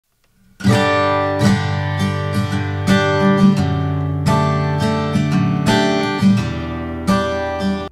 Embossed Rolling Pin
Cylindrical embossing on a rolling pin using the Emblaser.